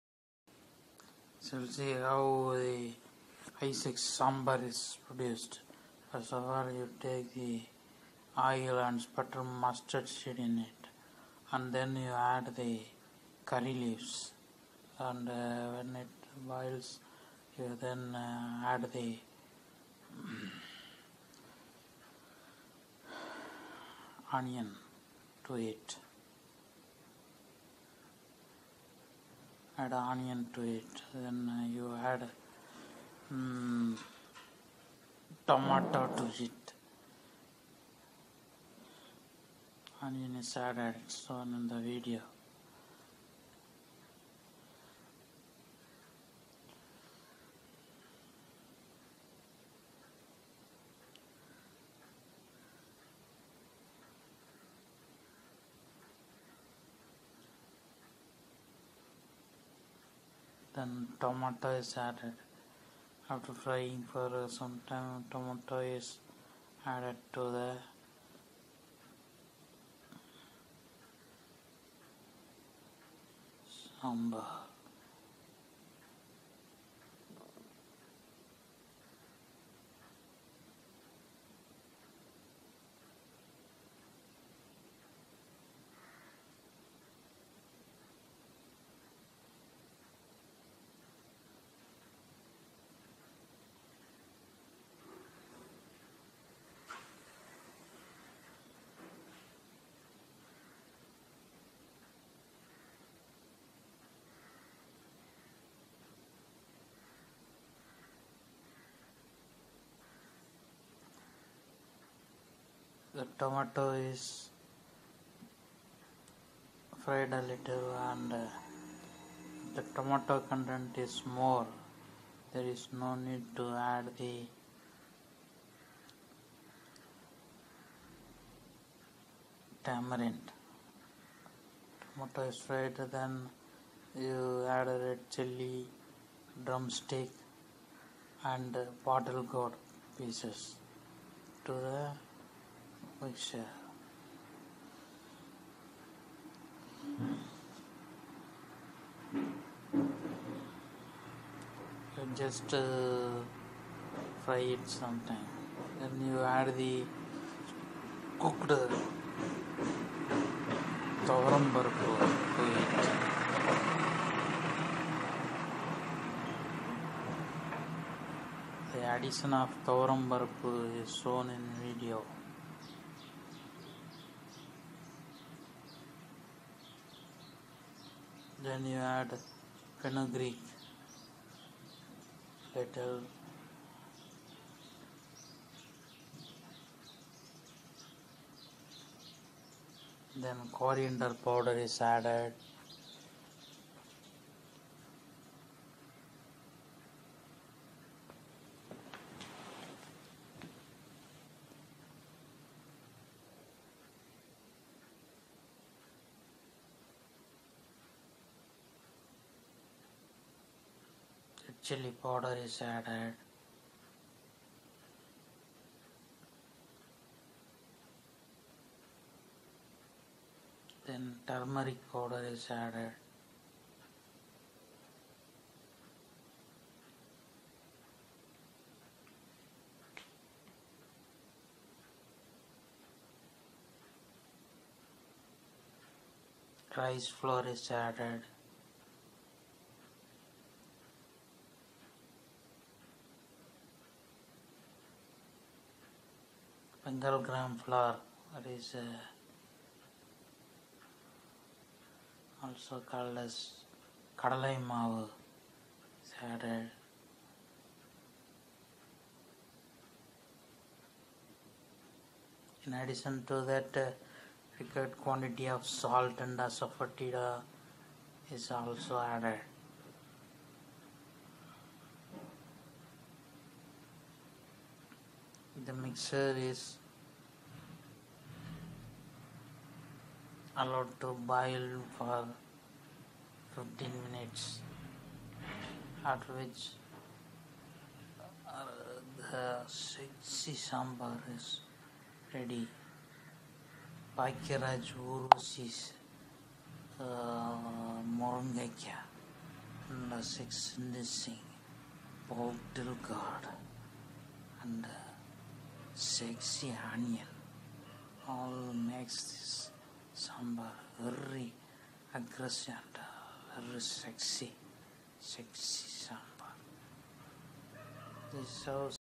High sex sambar drumstick onion bottle gourd in english
Making of high sex sambar is shown